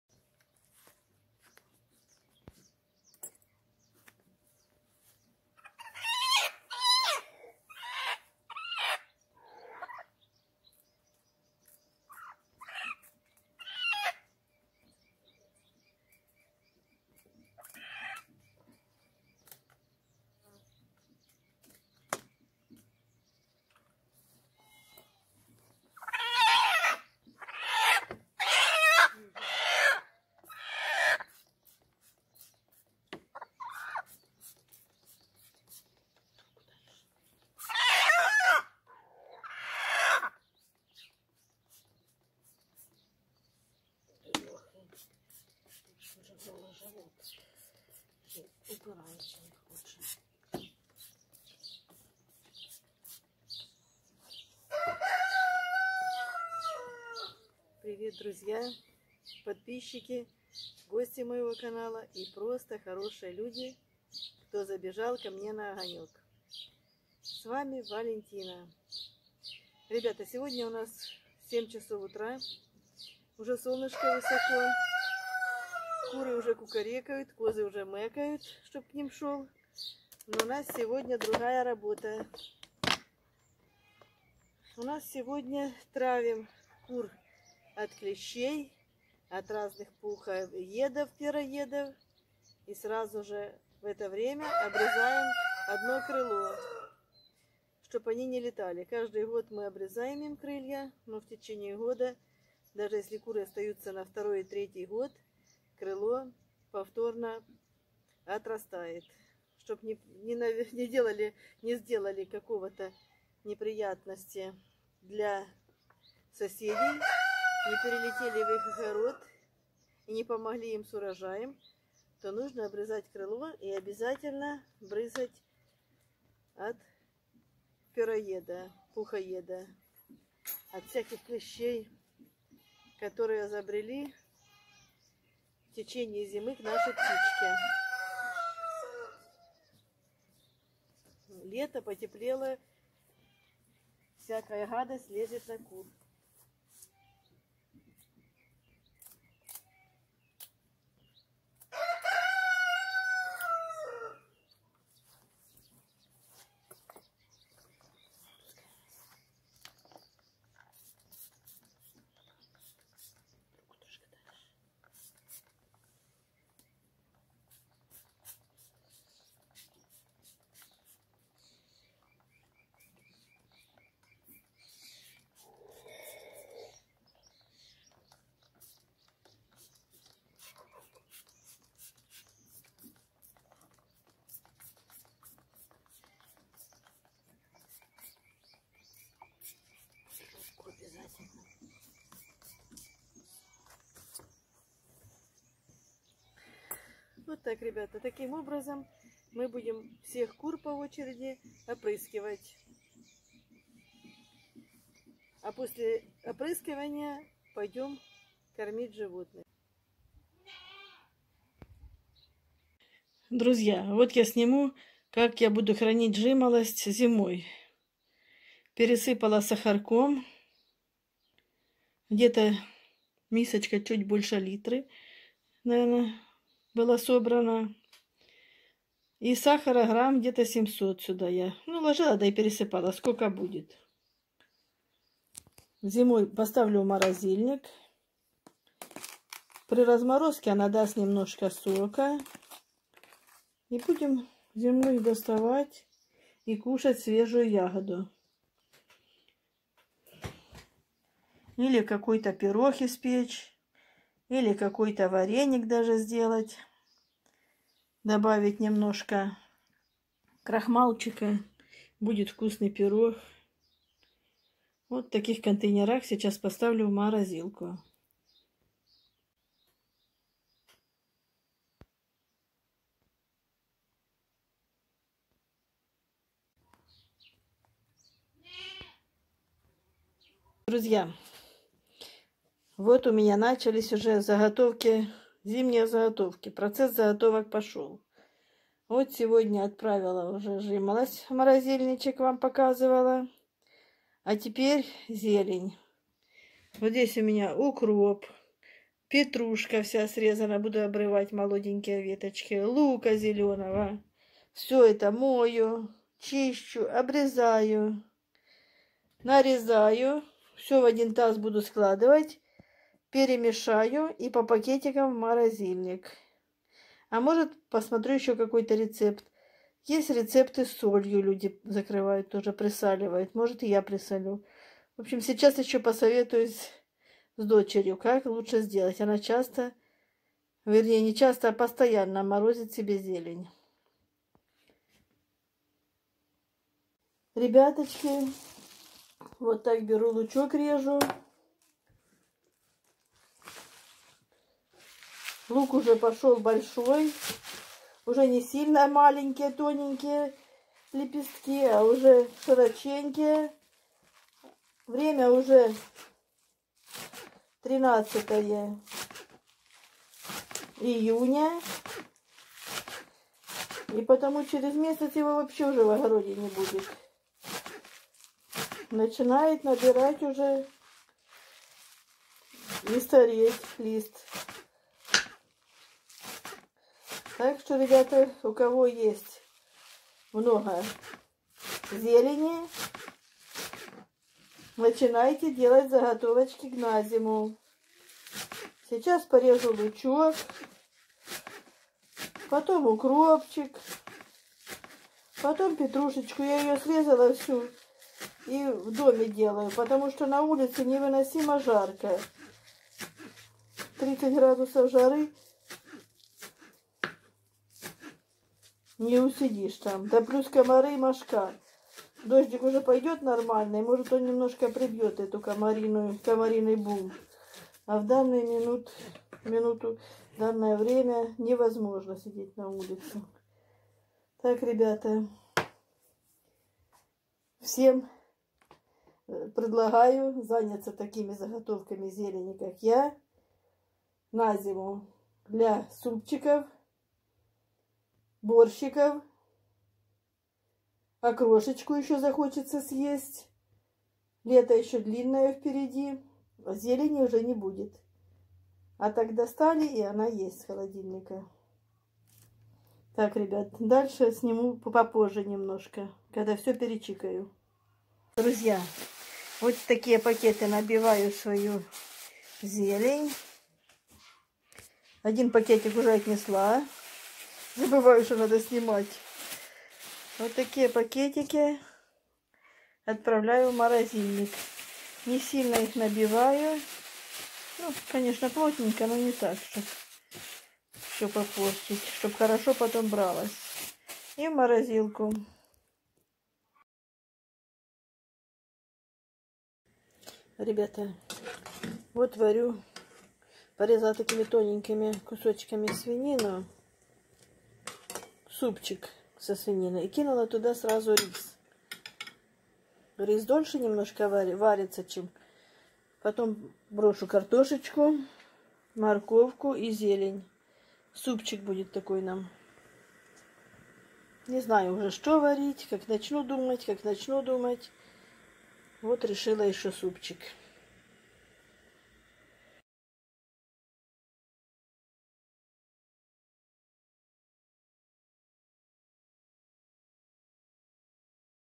ЖИЗНЬ В ДЕРЕВНЕ / салон красоты у../заготовки на зиму пошли /супчик со свининой/ заготавливаю зелень
ЖИЗНЬ В ДЕРЕВНЕ /утренний салон красоты у../заготовки на зиму пошли/супчик со свининой/морожу зелень